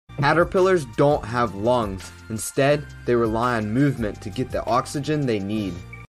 Can Caterpillars BREATHE?! || New Year Animal Shorts (part 7 of 31)
Caterpillars are different than most animals. One such way is their unique way of getting oxygen.